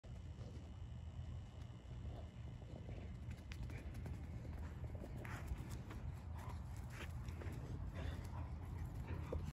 Video of adoptable pet named Finn - MS
Finn - MS is an adoptable pet with American Belgian Malinois Rescue, Inc in Stevens Point WI.